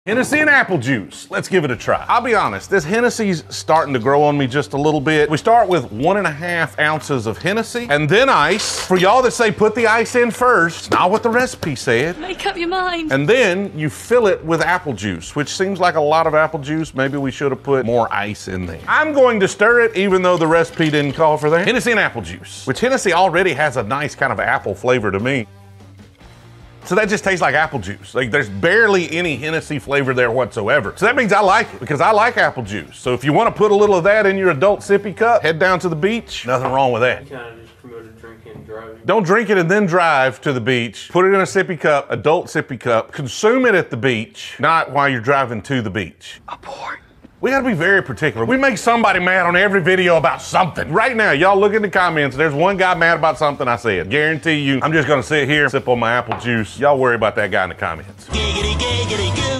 Henny & Apple Juice!
RECIPE:
1 1/2 oz of Henny
ICE
FILL WITH APPLE JUICE

Make sure to use an ADULT sippy cup.. also, don't drink and drive.

MMMMM... GOOD.